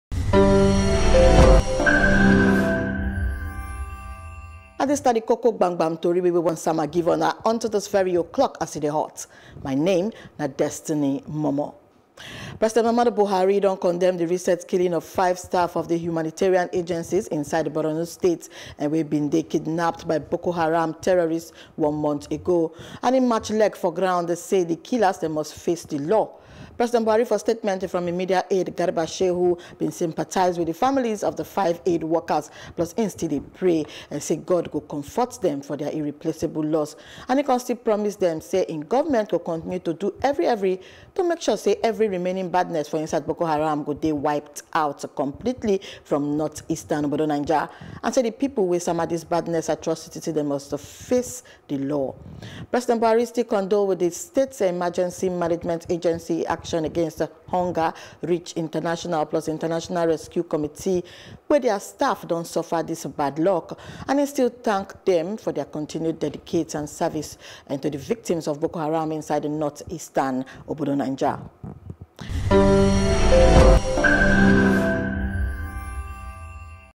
Pres. Buhari Yan Say Killers of Borno Aid Work Pepo Go Face Law
Oga Buhari Yan Says Killer of Borno Aid Work Pepo Go Face Law